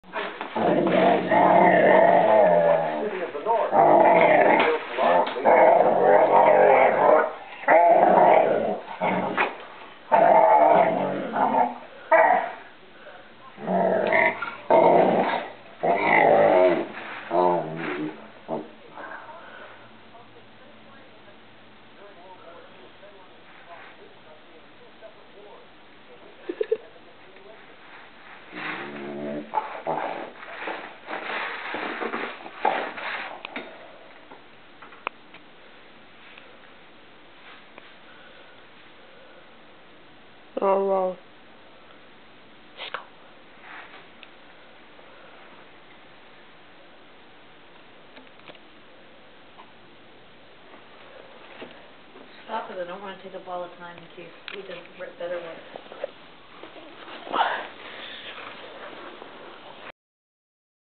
Cisco the Funny Viszla 2
Cisco having a really short shnoz.